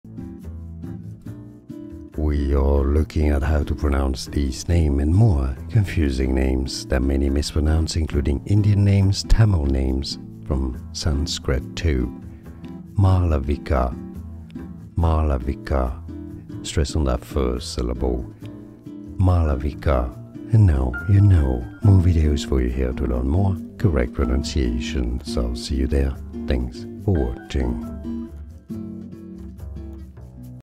How to Pronounce Malavika
This video shows you How to Pronounce Malavika, pronunciation guide.

00:00 - Pronunciation Intro
00:07 - How to Pronounce Malavika
00:20 - More Difficult Pronunciations